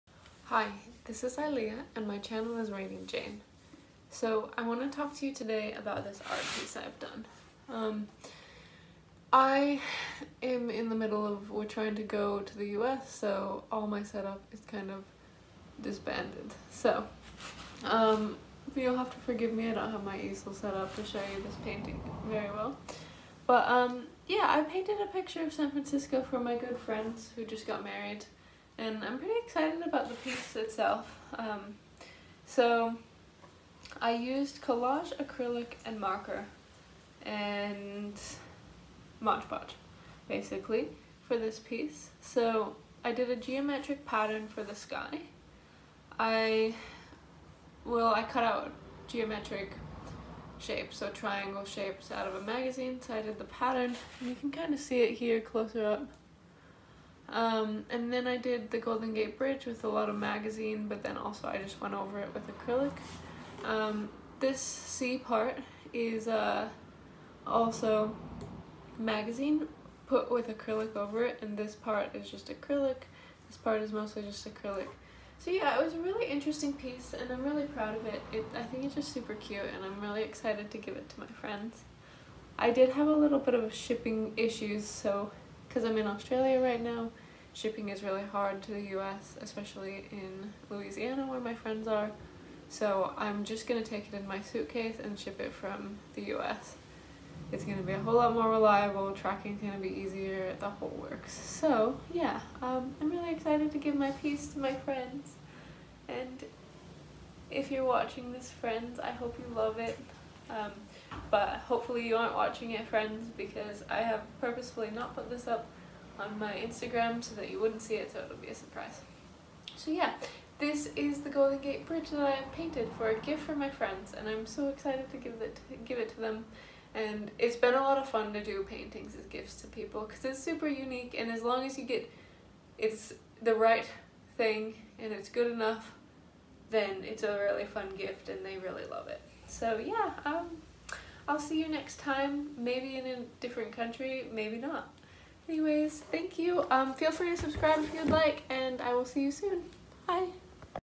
Latest Painting Show and Tell
This is my latest painting that I’m taking with me in my suitcase from Australia to the USA. It’s a wedding gift for some friends who just got married. Hope they love it! 😊